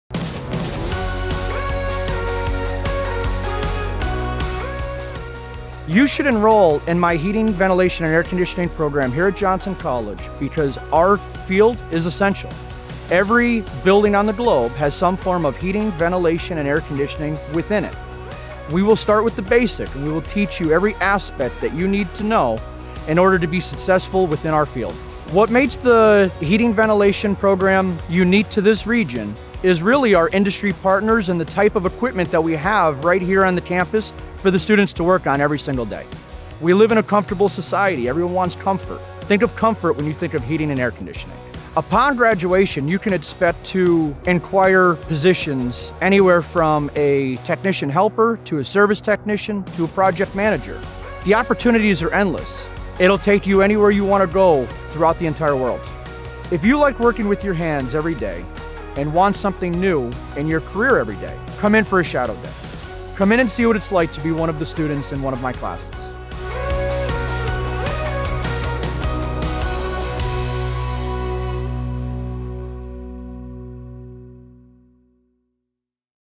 Johnson College - Heating, Ventilation & Air Conditioning Technology,
Walter Wood '11, Program Director, Heating, Ventilation & Air Conditioning Technology, talks about his program at Johnson College and why it might be the right fit for you.